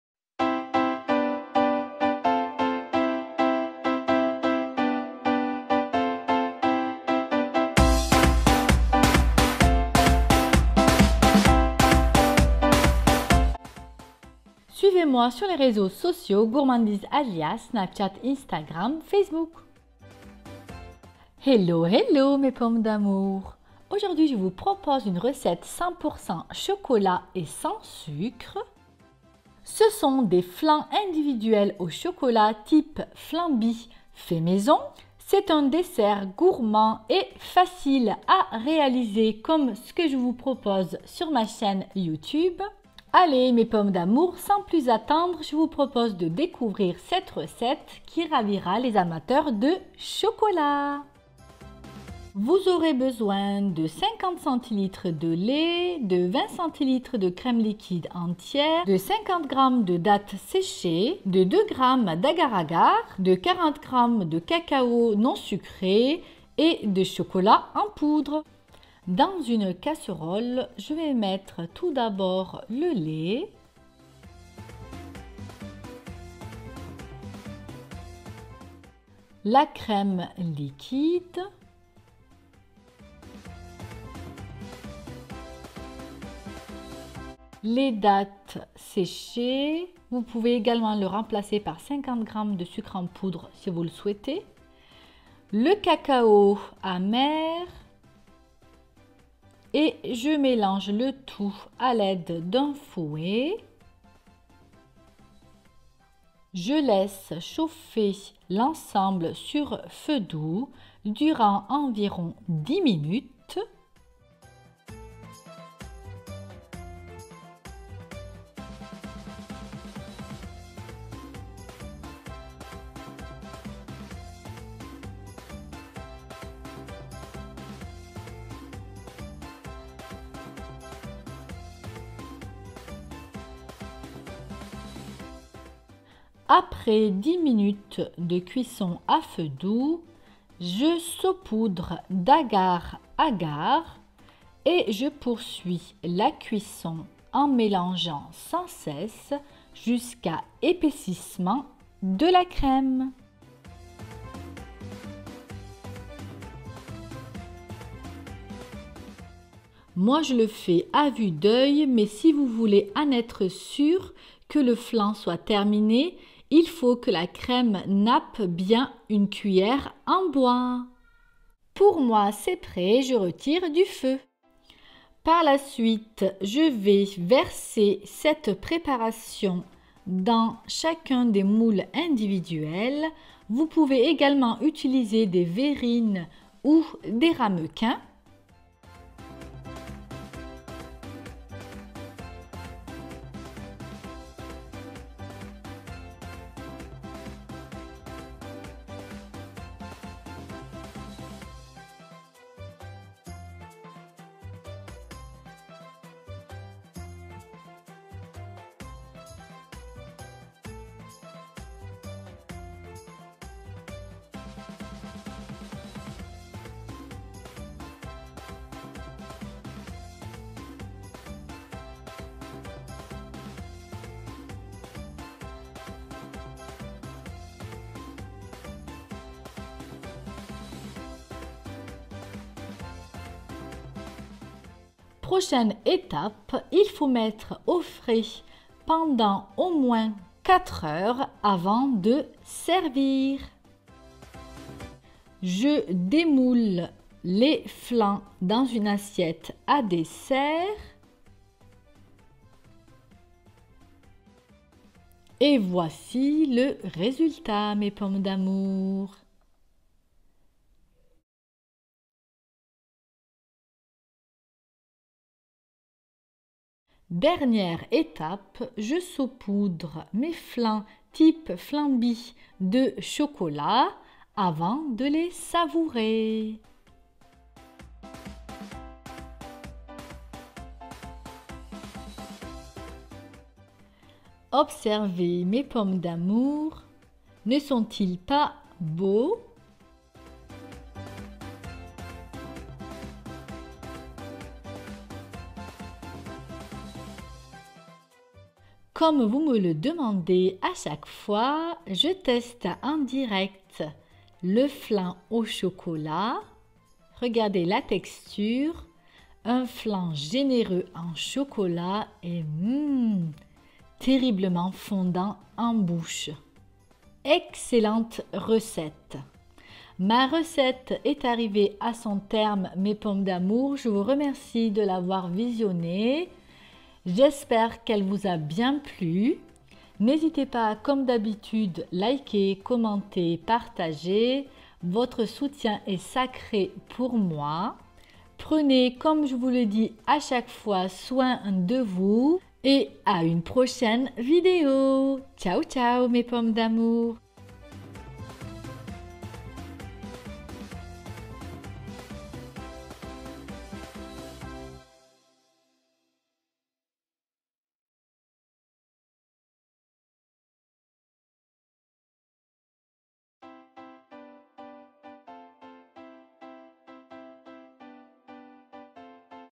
🍎❤️COMMENT FAIRE DES FLANS AU CHOCOLAT SANS SUCRE TYPE FLAMBY - RECETTE FACILE
Je vous propose la recette des petits flans au chocolat express, sans sucre, recette facile.

🎈Ce dessert facile et rapide est une recette classique et incontournable. C’est un dessert parfait pour commencer à apprendre la pâtisserie aux enfants.

La recette est simple et rapide pour un résultat onctueux et savoureux.

À TESTER D’URGENCE ❤️❤️❤️

Ingrédients pour 6 personnes  :

-25cl crème liquide entière

-50cl lait

-50g dates séchées (ou 50g sucre en poudre)

-2g d’agar agar

-40g cacao non sucré

-50g chocolat pâtissier

Mettre la crème liquide, le lait, les dates séchées ainsi que le cacao non sucré dans une casserole. Chauffez pendant 10 minutes à feu doux. Ajoutez l’agar l’agar, mélangez et poursuivez la cuisson en mélangeant pendant 10 minutes (jusqu’à apaississement). Versez dans les moules en silicone individuels ou des ramequins. Réservez au frais durant 4 heures . Démoulez et saupoudrez de chocolat avant de servir.

Bon appétit

LIKEZ, COMMENTEZ ET PARTAGEZ MA VIDÉO.

N’hésitez pas à me suivre sous Instagram Facebook et Snapchat en cliquant sur les liens ci-dessous :